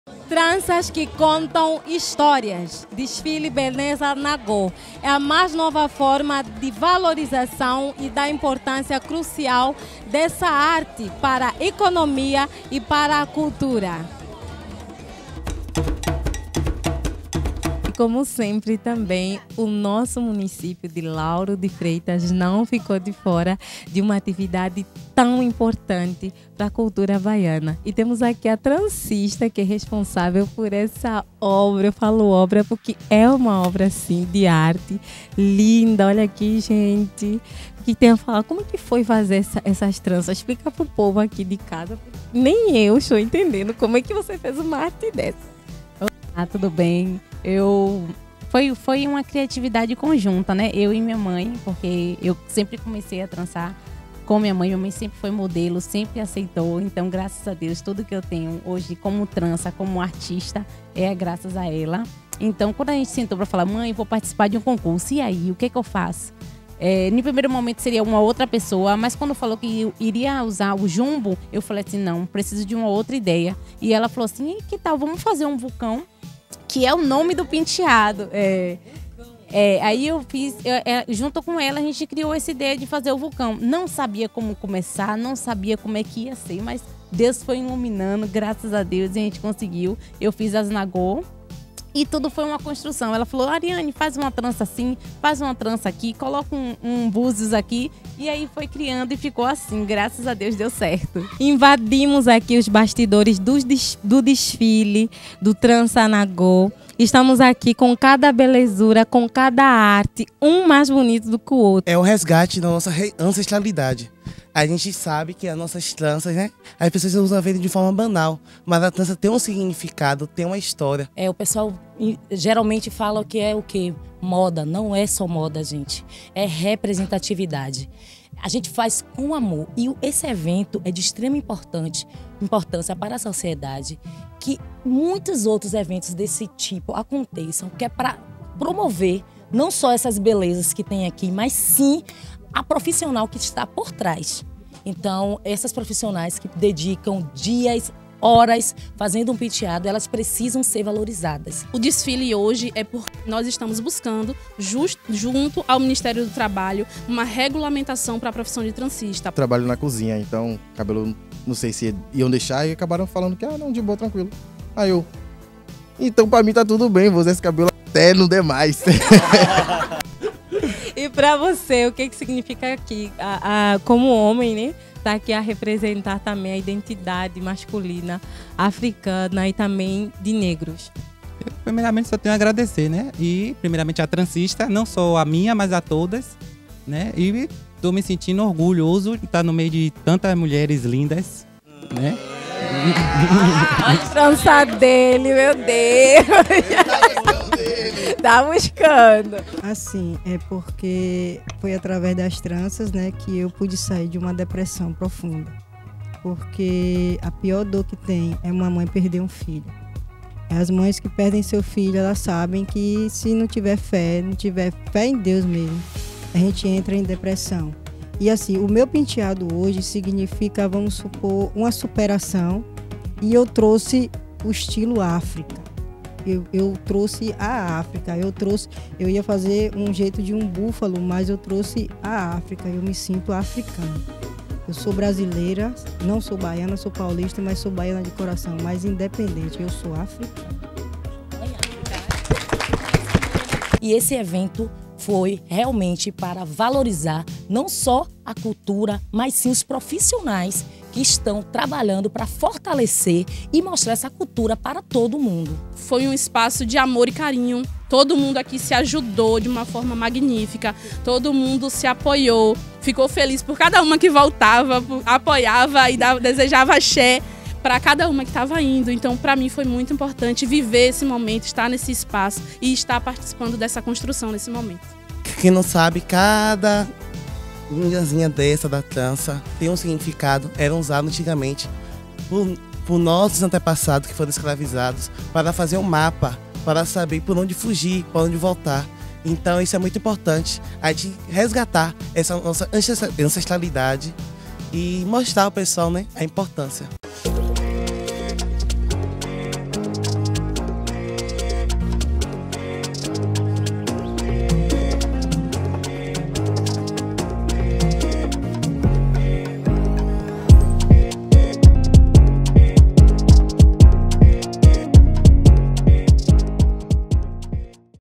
Concurso de trança Nagô - parte 02
No último sábado (25), o centro de cultura da câmara de vereadores de Salvador, foi palco de um verdadeiro espetáculo de artes.
Só que desta vez as verdadeiras artistas foram as profissionais de “Tranças Nagô” que apresentam a beleza em forma de arte, cultura, identidade e resistência.

No mês da consciência negra, aprendemos muito mas através deste primeiro concurso de “Beleza Nagô” que foi realizado pela presidente de comissão de defesa dos direitos mulheres, vice da comissão de repartição, vereadora Ireuda Silva (republicano).

Não são apenas tranças é a nossa história, de reafirmação, valorização cultural e econômica.

Desta forma parabenizamos a todos os participantes deste evento que faz renascer a voz de pessoas negras através da arte e da cultura.